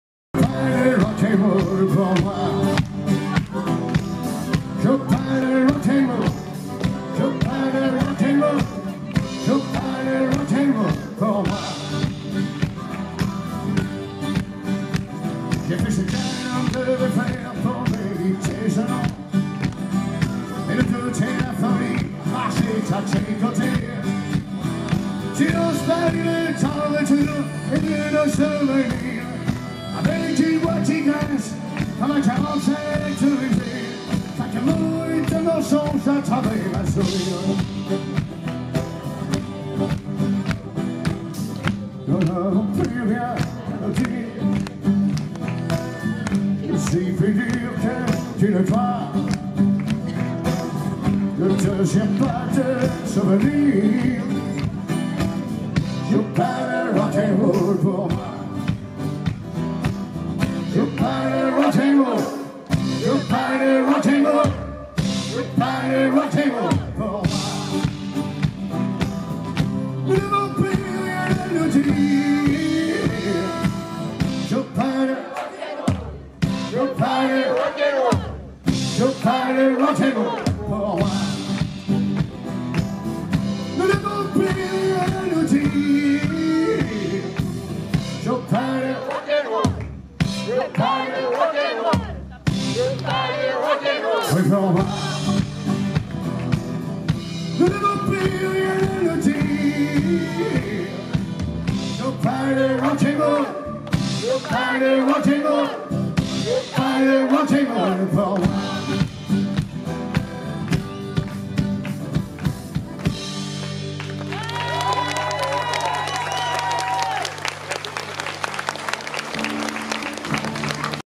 Concert Jean Baptiste Guégan (Partie 12) 2018
Sosie Vocal de Johnny Hallyday, concert hommage.

Tournée Partouche à Ronce Les Bains.
25 Juillet 2018.